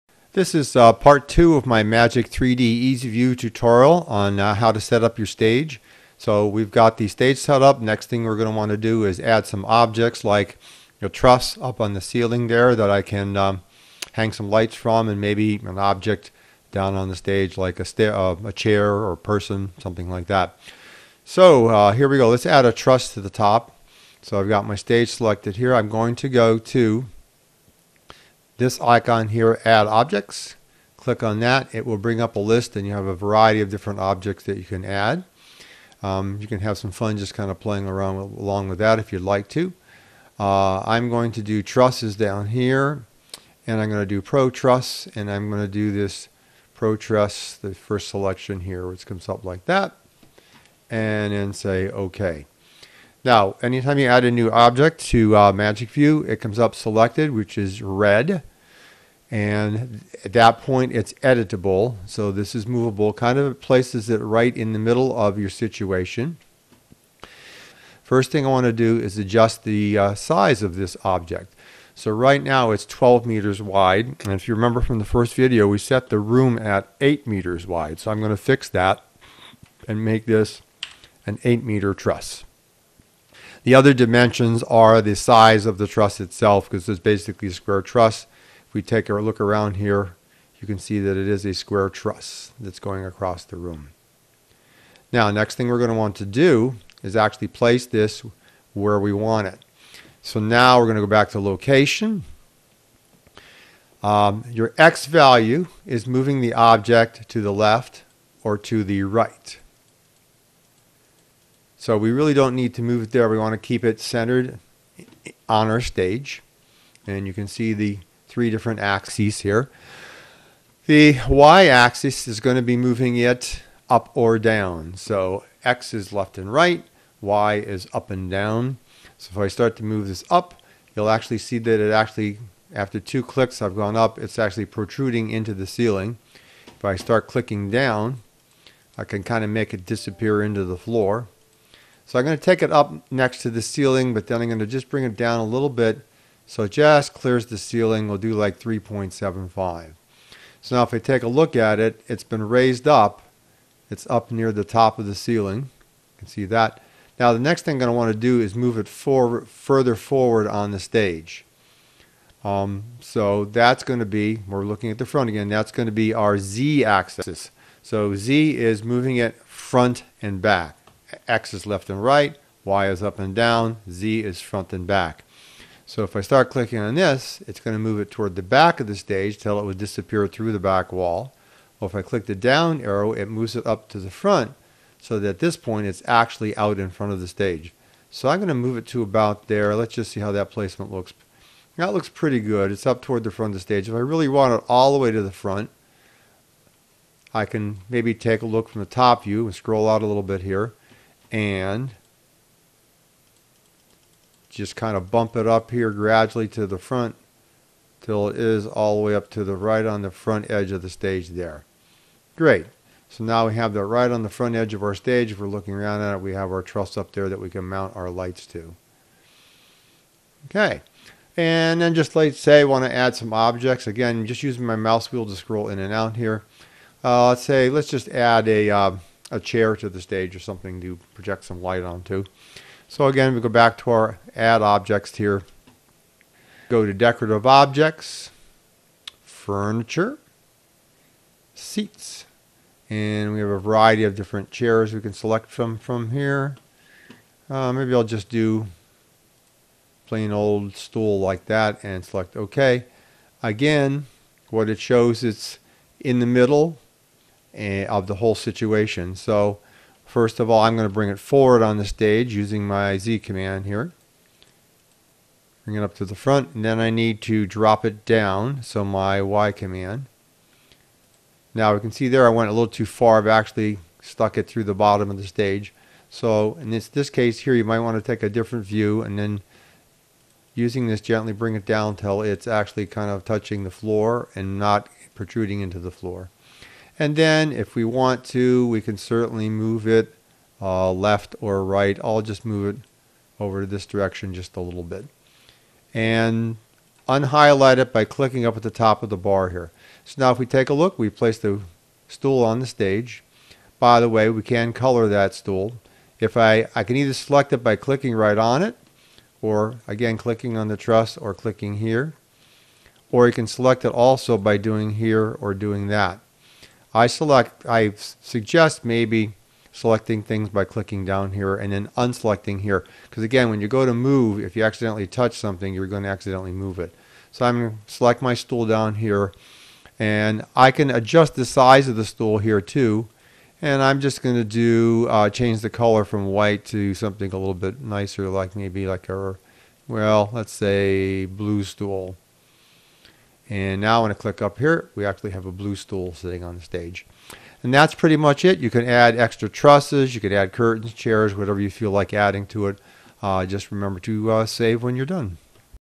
QLC+ Visualizer Part2 Objects
In this video I show how to place objects into the visualizer. Magic 3D Easy View is software which is a 3D visualizer program that allows you to create a 3D stage design with light fixtures that project light beams. These fixtures and light beams are in turn controlled by the QLC+ software. It is a way to visualize your stage lighting setup in 3D without actually being at the stage or theater. Dimensions, fixtures and light beam size and shape are accurately represented so you can use this to determine the placement and type of lighting fixture you might use in designing your lighting plan for a production.
This series of videos was created to help community theater groups. It is an introduction and series of tutorials using QLC+, a lighting program that can replace a traditional light board and is a great alternative to community theater groups due to its attractive cost.

This is fabulous software developed by Massimo Callegari and other contributors.